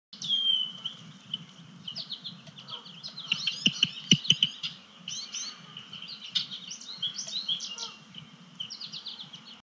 5 11 2019 American Mink
American Mink scurries across the Berm at Sapsucker Woods Pond.